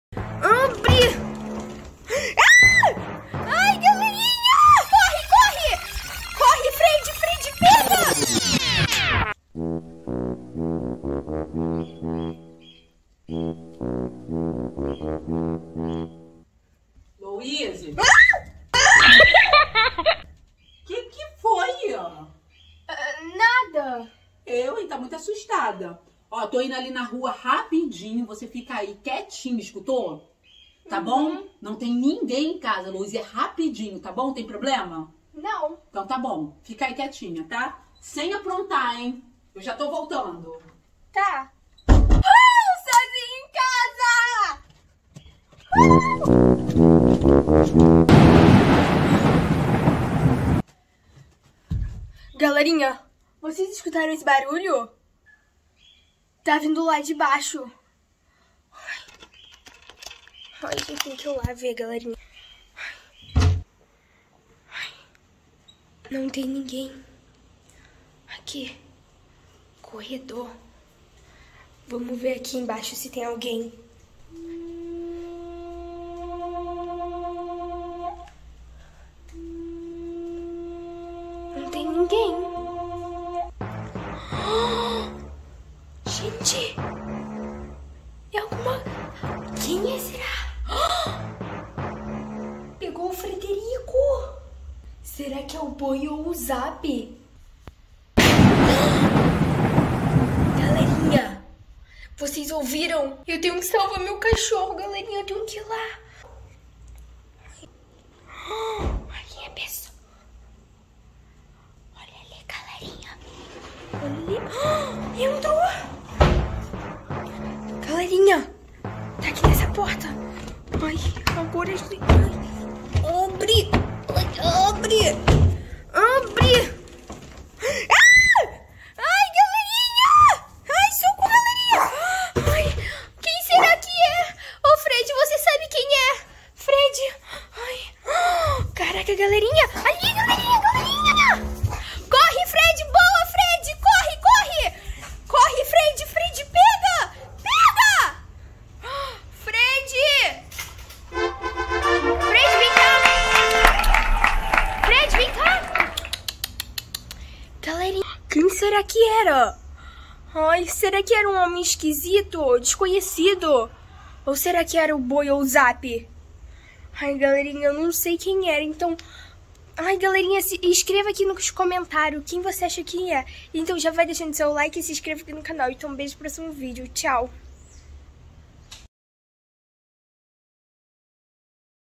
A LOUISE FICOU SOZINHA E INVADIRAM NOSSA CASA SERÁ O BOI OU ZAP?
A LOUISE FICOU SOZINHA EM CASA E INVADIRAM NOSSA CASA E PEGARAM NOSSO CACHORRO. SERÁ O BOI OU ZAP?